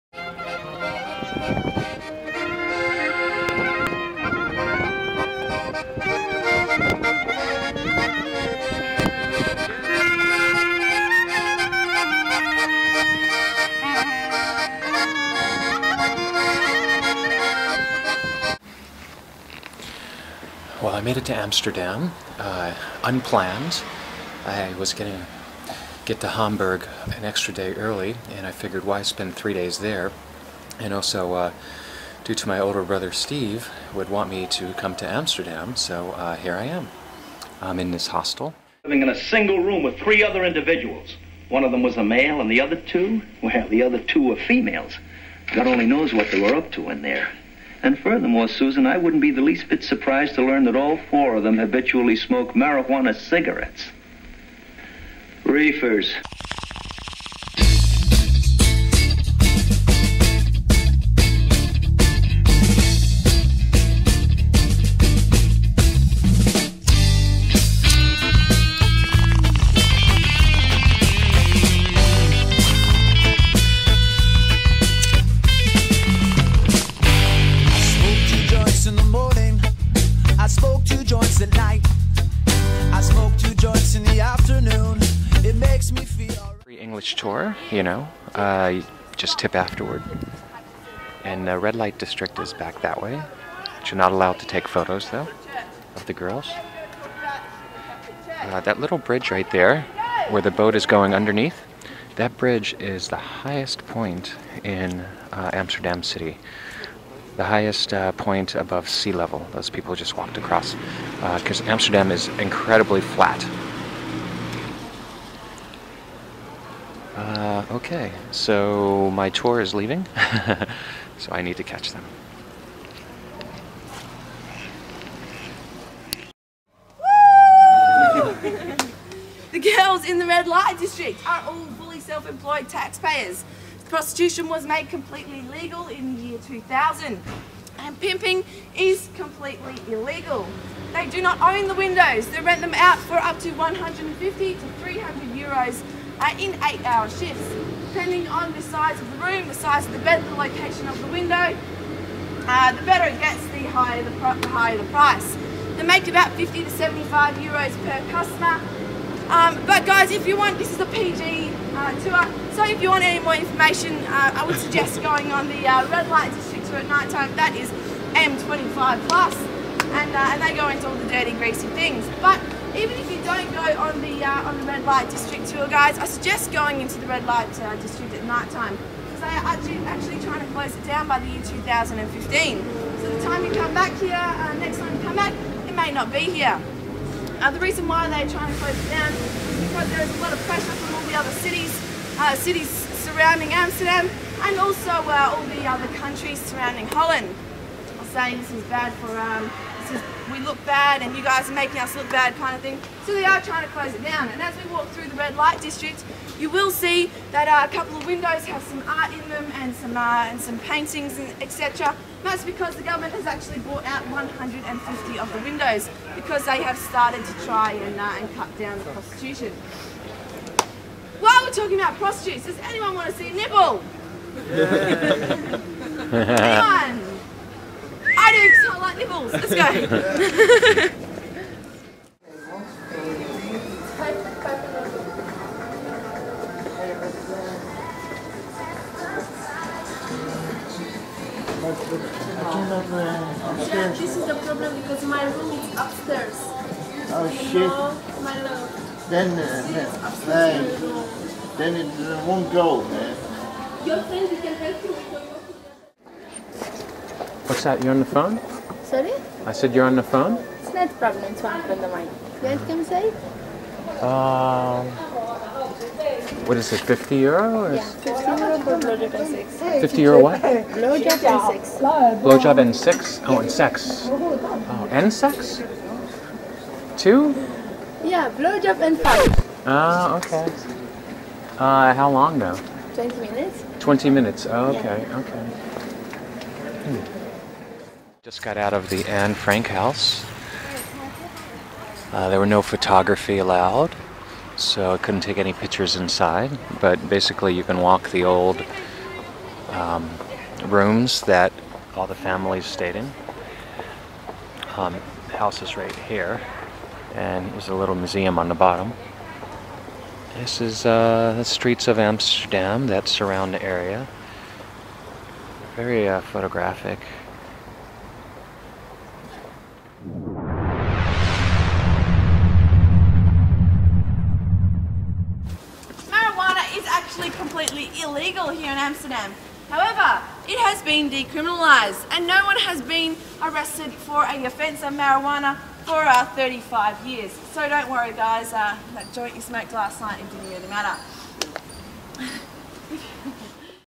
VLOG 0910F: AmsterDAMN
An unscheduled overnight on the world tour. Rob investigates the fleeting sins of this international city, but of course cannot partake.